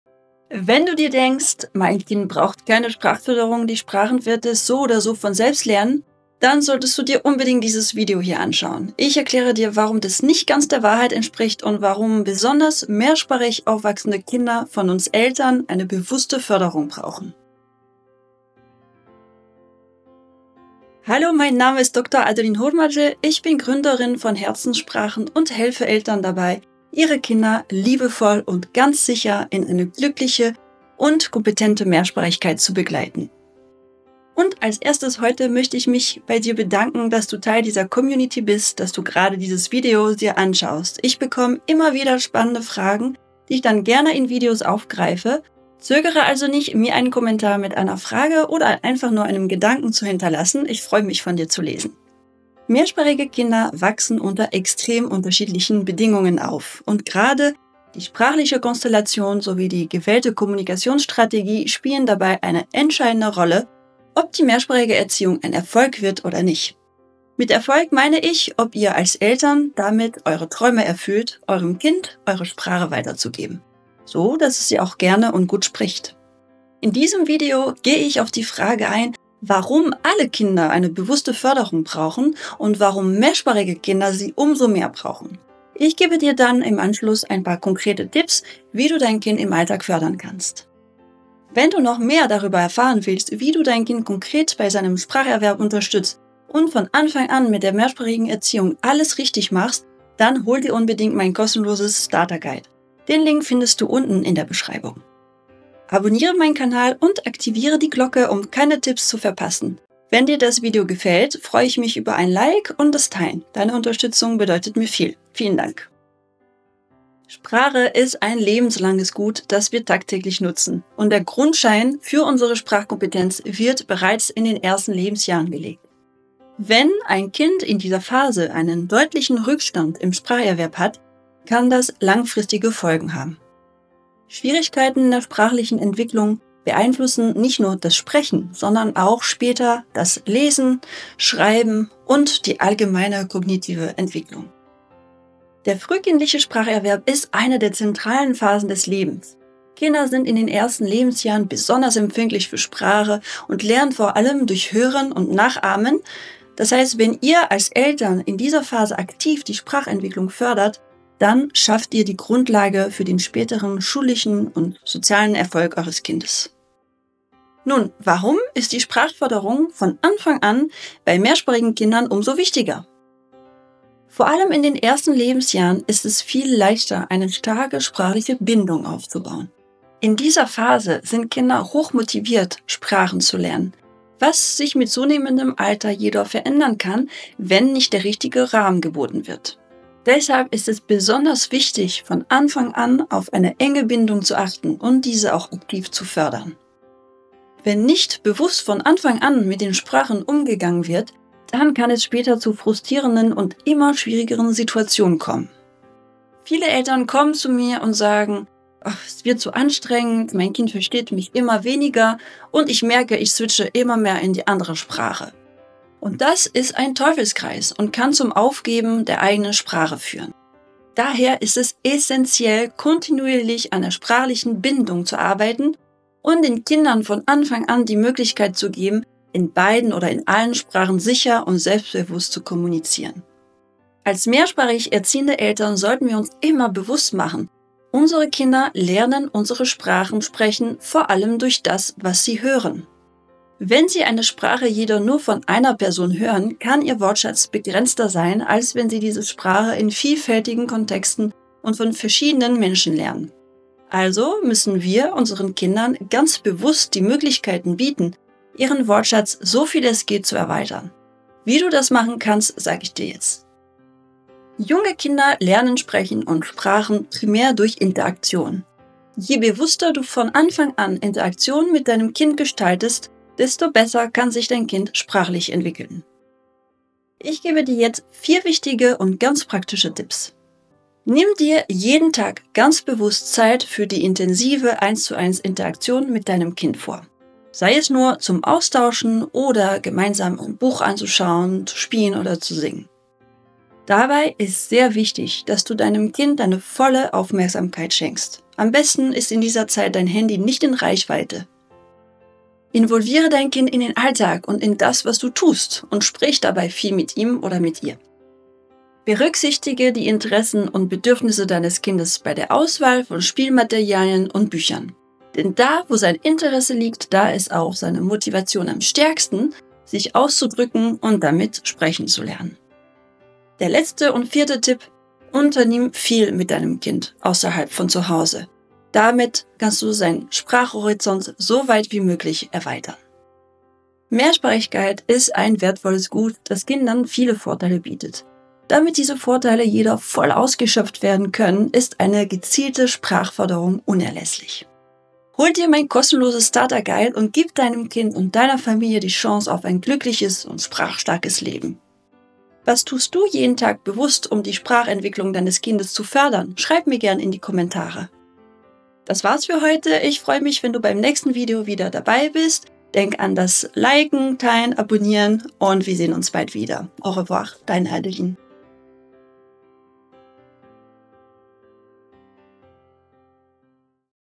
Sprachförderung: Warum sie bei mehrsprachigen Kindern besonders wichtig ist! bilingual aufwachsen
Mehrsprachigkeit ist ein Geschenk, das Kindern viele Türen öffnen kann – doch ohne gezielte Sprachförderung verpassen sie möglicherweise die Chance, das volle Potenzial ihrer Sprachkompetenz zu entfalten. In diesem Video erkläre ich dir, warum besonders mehrsprachig aufwachsende Kinder eine bewusste Sprachförderung brauchen und was du als Elternteil tun kannst, um dein Kind optimal in seiner Sprachentwicklung zu unterstützen.

Ich bin Dr. Adeline Hurmaci, Gründerin von Herzenssprachen, und begleite Eltern dabei, ihre Kinder liebevoll und sicher in die Mehrsprachigkeit zu führen. Wir besprechen, warum frühe Sprachförderung so wichtig ist und welche Herausforderungen es bei mehrsprachigen Kindern gibt. Außerdem teile ich wertvolle Tipps, wie du im Alltag die Sprachentwicklung deines Kindes förderst.

🌟 Hol dir meinen kostenlosen Starter Guide für einen gelungenen Start in die mehrsprachige Erziehung!

Wenn dir das Video gefällt, freue ich mich über ein Like und das Teilen mit anderen Eltern.

Kapitel:
00:00 – Einführung
00:52 – Warum ist Sprachförderung wichtig?
01:57 – Spezielle Herausforderungen bei mehrsprachigen Kindern
04:37 – 4 Tipps für die Sprachförderung im Alltag
05:34 – Fazit